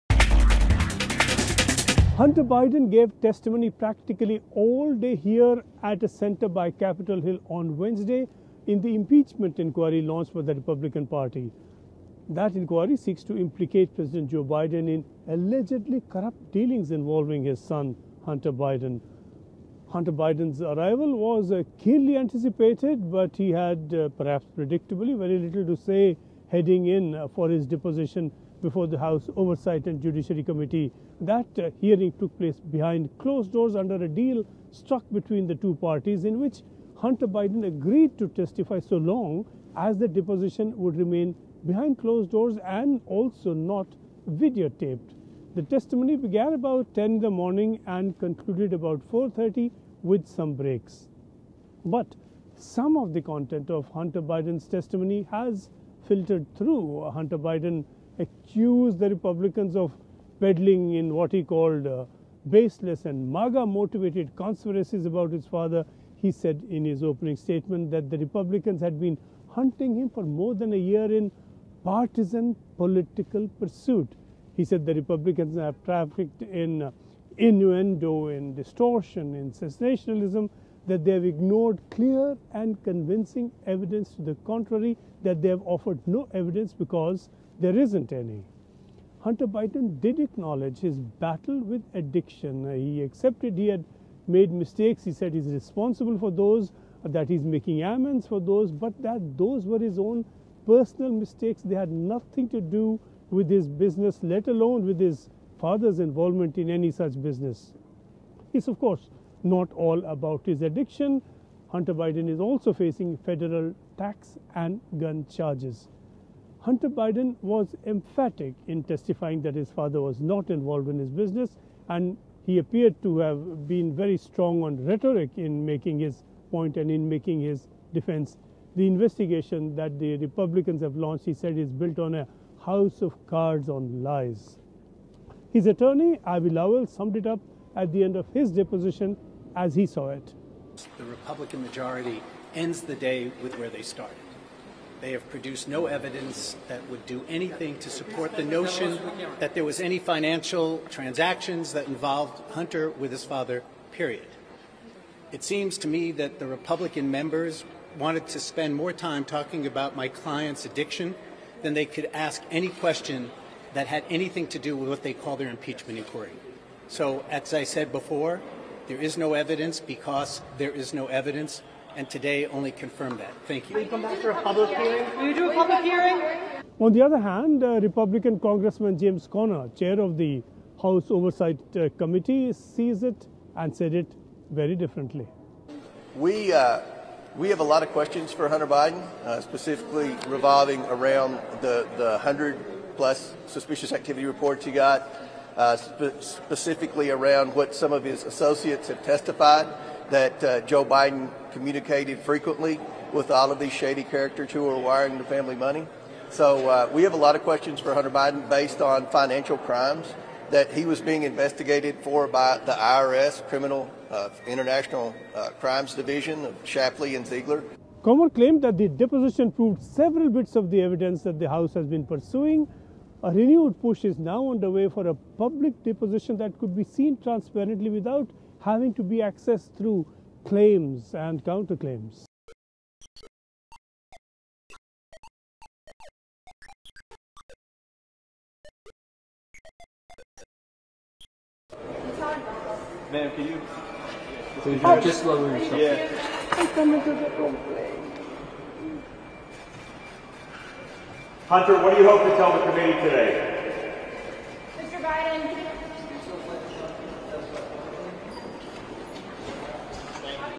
Hunter Biden Testifies | GOP Impeachment Inquiry | 'Joe Biden Was Never Involved In Business' | N18V
Joe Biden's son, Hunter, testified to a congressional impeachment inquiry against the President, telling Republicans he did not involve his father in his business dealings. Hunter Biden lashed out at what he called a 'baseless' investigation fuelled by 'conspiracy theories'. He said his testimony 'should put an end to this baseless and destructive political charade'. Lawmakers are looking into whether Joe Biden benefited from any corruption. President Biden has denied any wrongdoing.